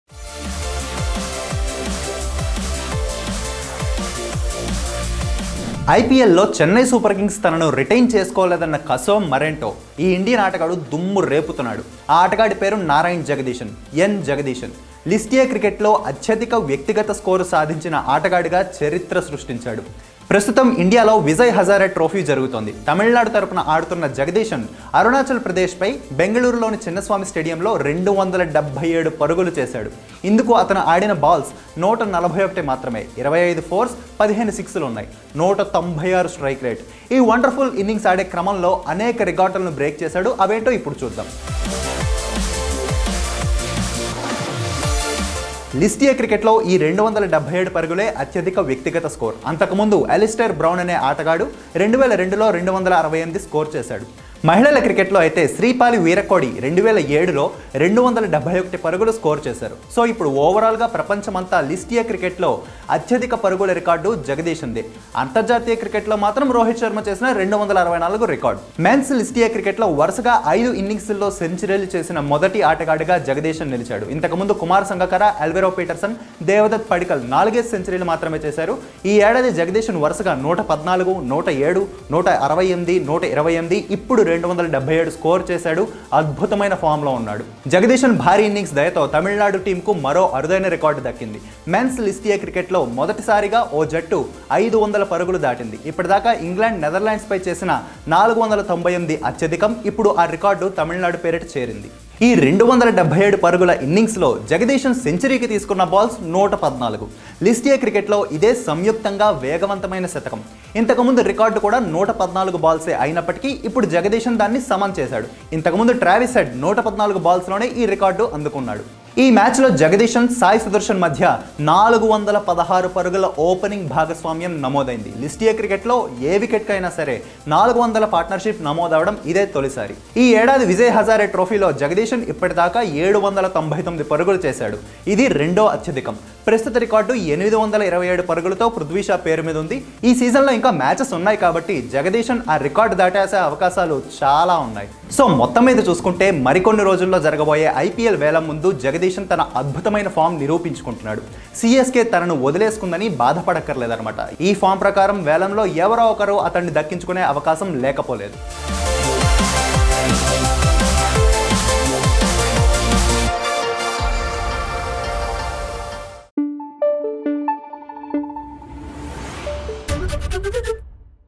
N Jagadeesan List A Cricket Records: వరుసగా 5 సెంచరీలతో రికార్డుల మోత మోగించిన జగదీశన్ | ABP Desam
ఐపీఎల్ లో చెన్నై సూపర్ కింగ్స్ తనను రిటైన్ చేసుకోలేదన్న కసే మరేంటో... ఈ ఇండియన్ ఆటగాడు... దుమ్ము రేపుతున్నాడు. ఆ ఆటగాడి పేరు నారాయణ్ జగదీశన్. ఎన్. జగదీశన్. లిస్ట్-ఏ క్రికెట్ లో అత్యధిక వ్యక్తిగత స్కోరు సాధించిన ఆటగాడిగా చరిత్ర సృష్టించాడు.